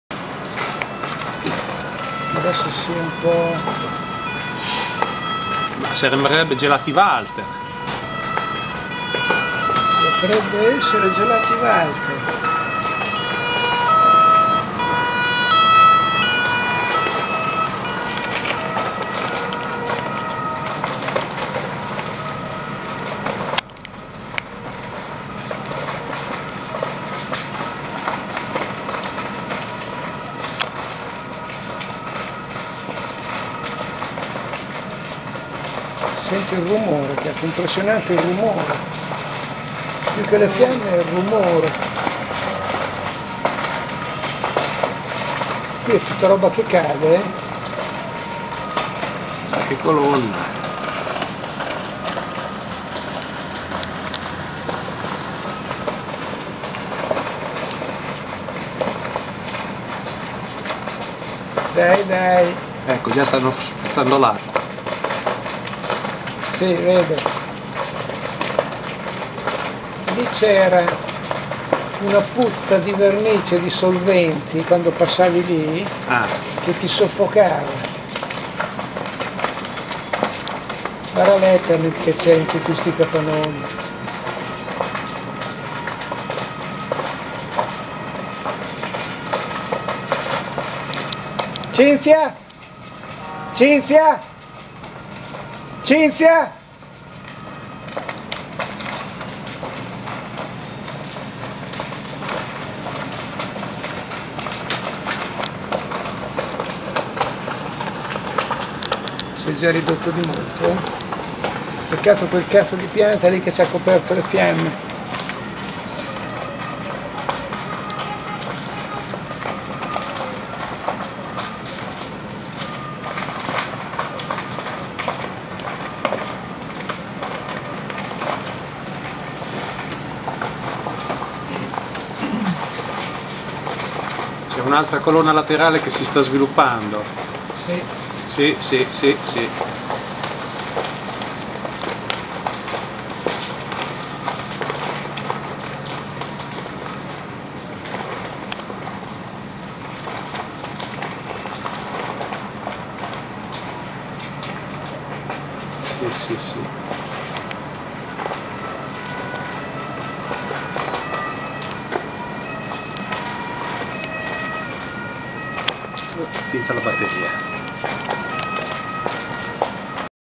Incendio a Pesaro 9 settembre 2011
Incendio a Pesaro: brucia il Mobilificio Gregori, in Via Gagarin. Ore 10.30 del 9 settembre 2011. Ripresa effettuata dal 4 piano di un edificio distante circa trecento metri